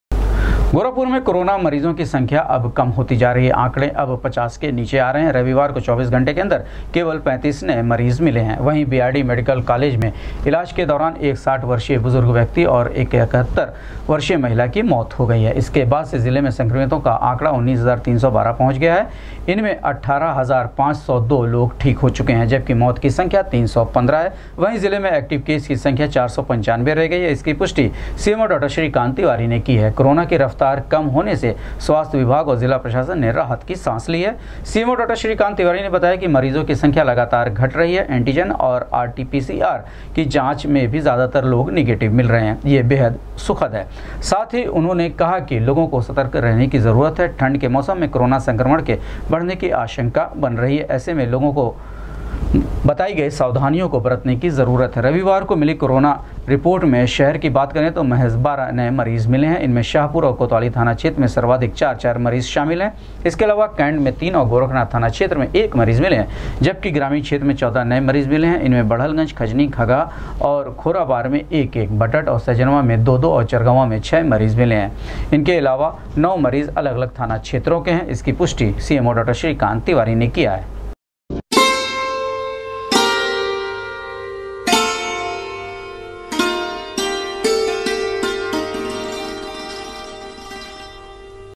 गोरखपुर में रविवार को मिले 35 नए कोरोना संक्रमित मरीज, एक कि हुई मौत, जिले में संक्रमित मरीजों की
गोरखपुर में रविवार को मिले 35 नए कोरोना संक्रमित मरीज, एक कि हुई मौत, जिले में संक्रमित मरीजों की संख्या हुई 19312, अब तक 315 कोरोना संक्रमित मरीज गवां चुके हैं अपनी जान, 495 हुए एक्टिव मरीज , सीएमओ डॉ श्रीकांत तिवारी ने की पुष्टि